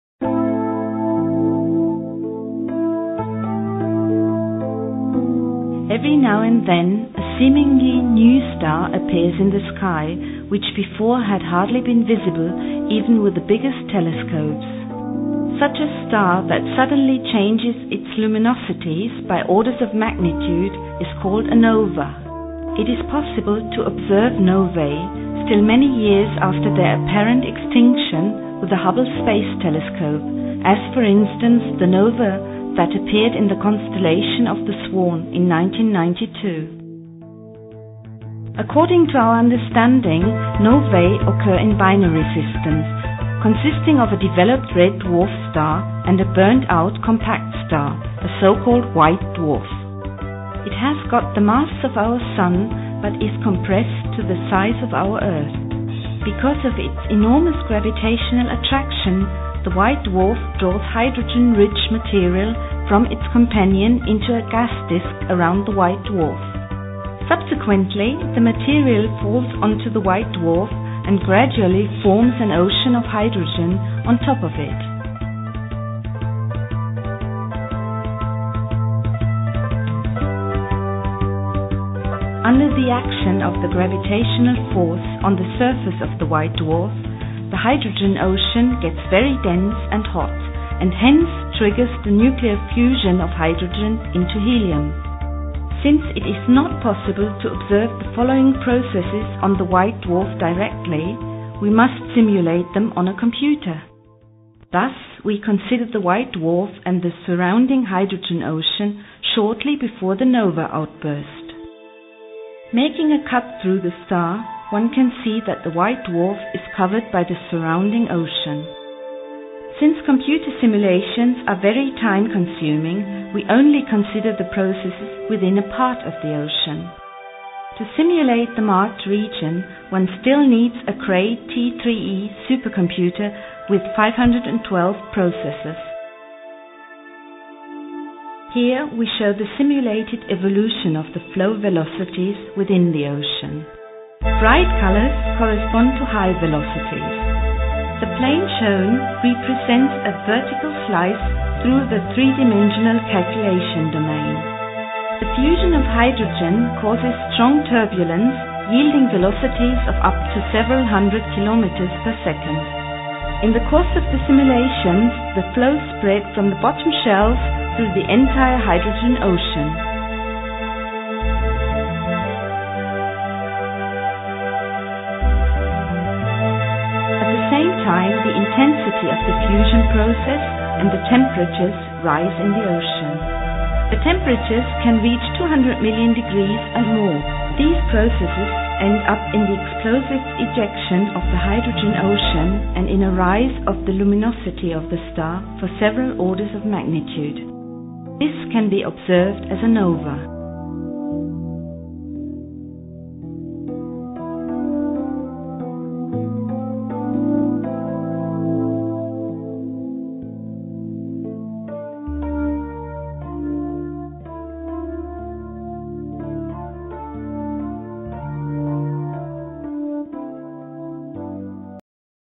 White Dwarf Star and Novae
Video from the 'Cosmic Cinema' series by the Max Planck Institute of Astrophysics regarding Nova (a cataclysmic nuclear explosion caused by the accretion of hydrogen from a neighbouring star onto the surface of white dwarf.